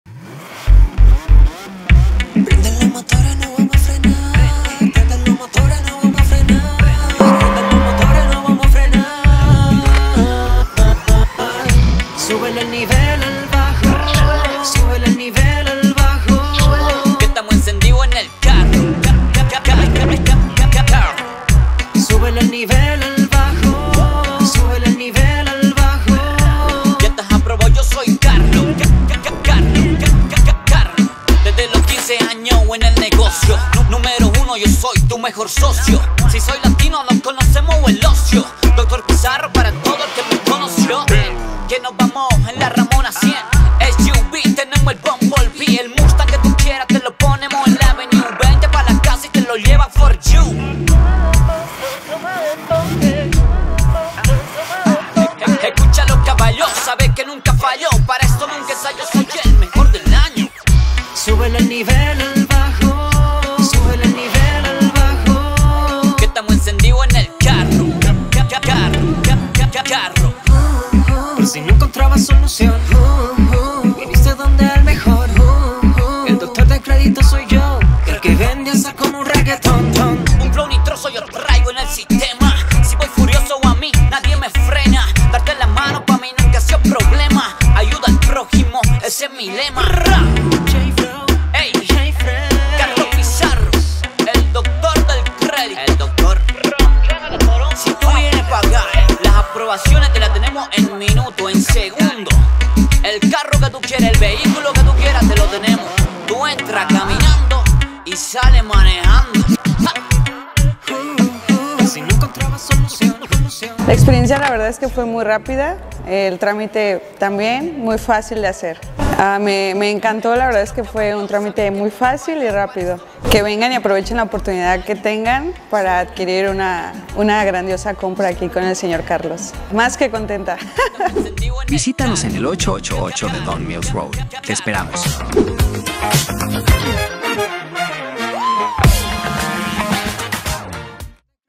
El trámite fue muy fácil de hacer - 2024 RAM 1500 Rebel - Doctor del Credito
Ven por tu 2️⃣0️⃣2️⃣4️⃣ RAM 1500 Rebel 🛻

Una experiencia muy rápida y el trámite fue muy fácil de hacer. Vengan y aprovechen la oportunidad de comprar un vehículo con Carlos Pizarro el doctor del credito. 🇲🇽📍🇨🇦